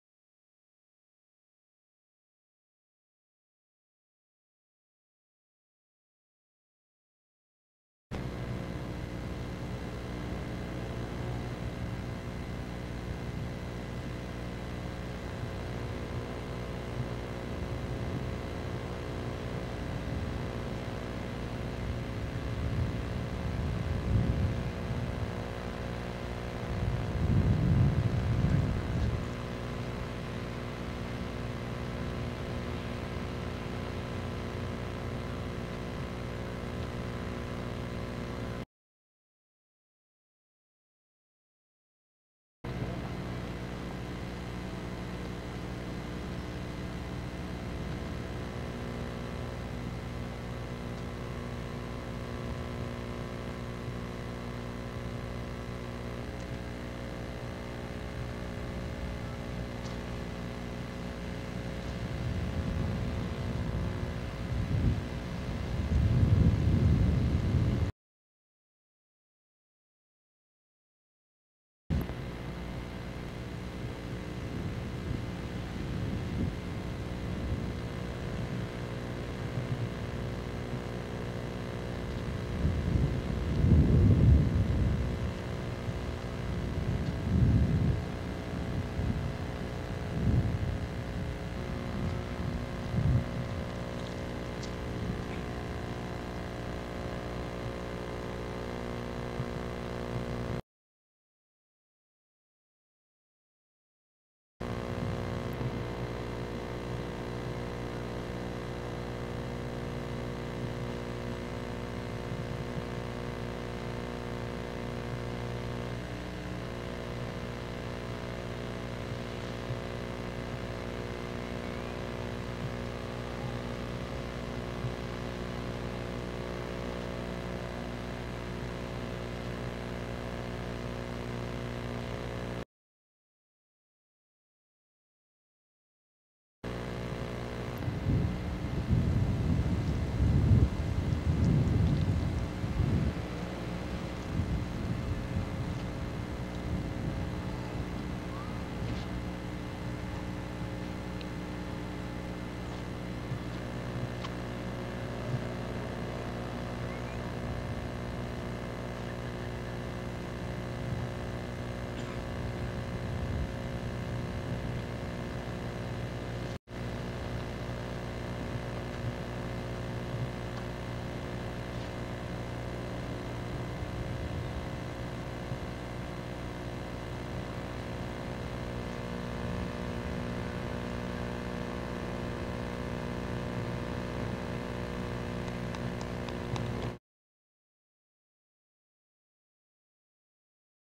Bing Gong performing Xing Yi Quan
This is a video of Bing Gong performing four of the five elements of Xing Yi Quan from Adam Hsu.  This was filmed by John Burns in the Golden Gate pan handle around the late 1980’s.  The Last section is the linking form. The video comes from Bing’s private collection.